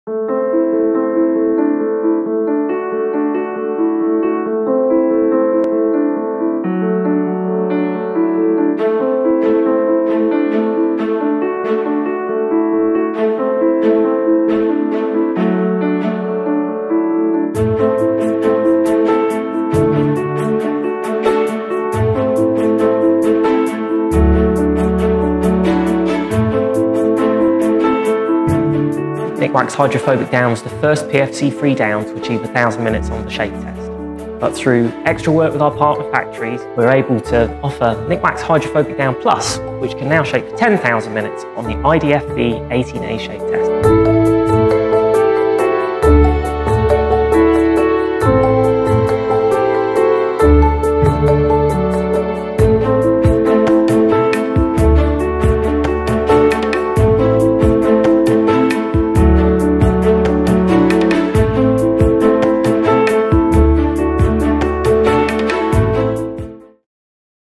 Wir stellen vor: NHD+
Die erste und einzige hydrophobe Daune mit 10.000 Minuten im Daunenschütteltest